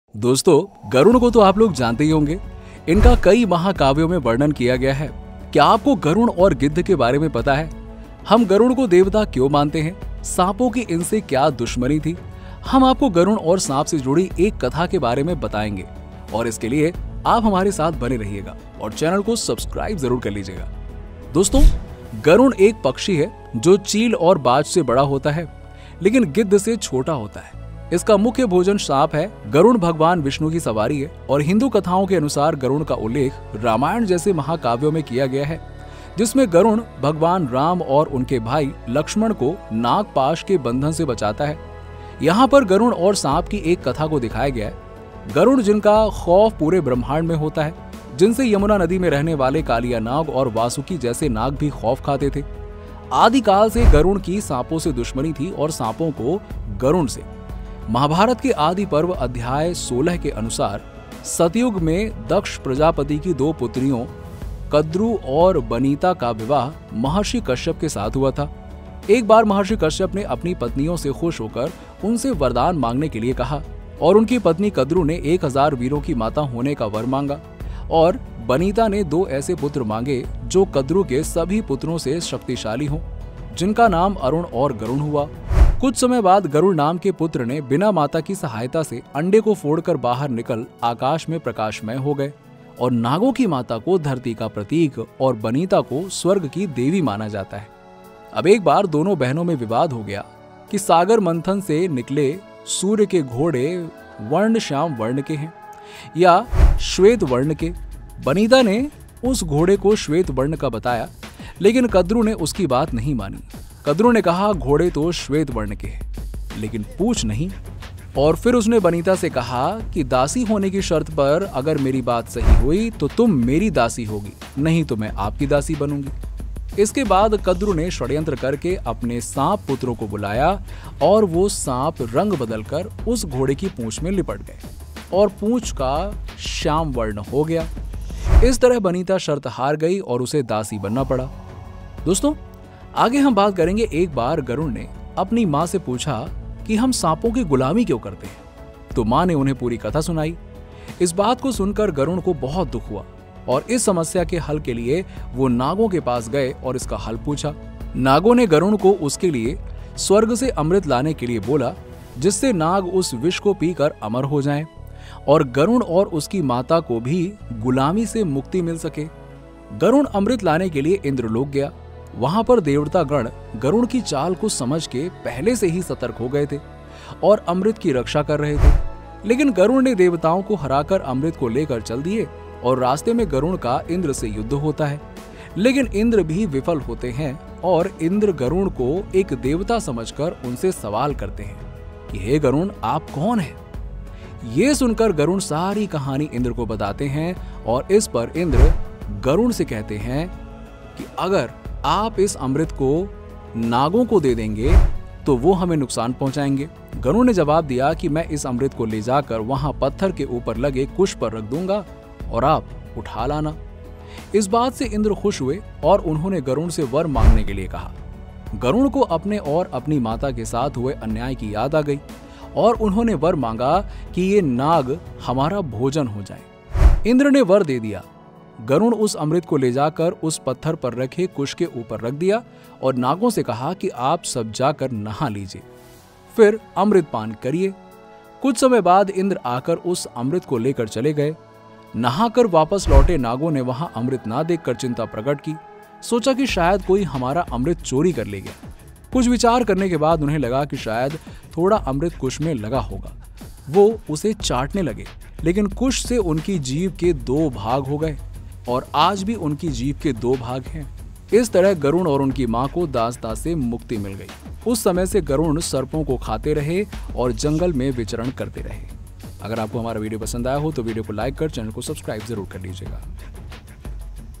क्यों हो गयी गरुड़ और नाग की शत्रुता ? Enmity Between Garuda and Naag | Story Of Naag And Garun
हिंदू मान्यताओं के अनुसार गरुड़ पक्षियों के राजा है और भगवान विष्णु का वाहन हैं। नाग और गरुड़ के बीच मतभेद सदियों से चला आ रहा है इनके बिच हमेशा ही शत्रुता रही  तो आये जानते है इसके पीछे की कहानी क्या थी गरुड़ और नाग की शत्रुता की मुख्य वजह क्या है